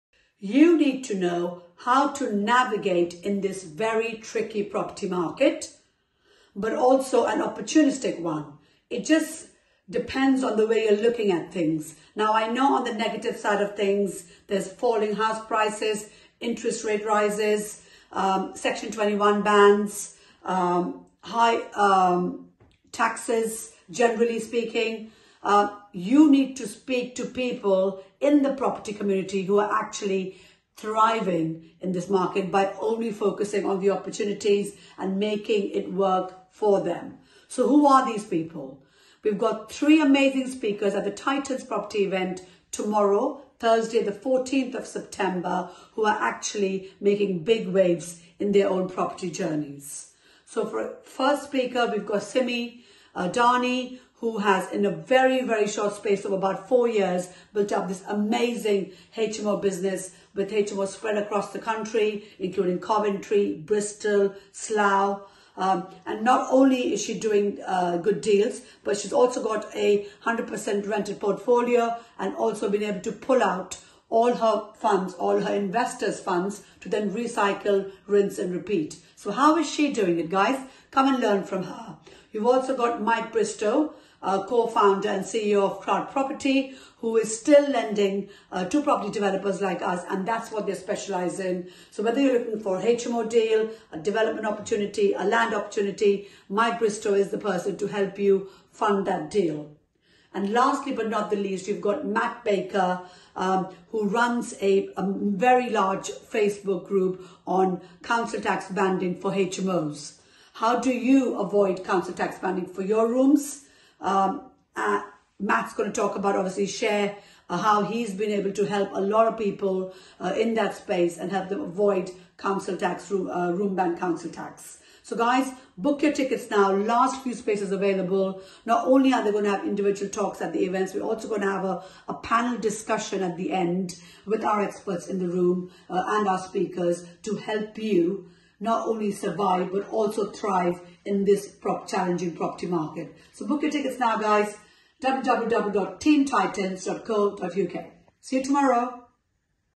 How are you NAVIGATING this tricky PROPERTY market?
With rising interest rates , high taxes, falling house prices , S21 ban looming and UK economy shrinking there is a lot of CHALLENGES. How do you FOCUS on the OPPORTUNITIES instead ?

Find out other property investors are doing at the TITANS event tomorrow Thursday 14th September. Last few spaces available!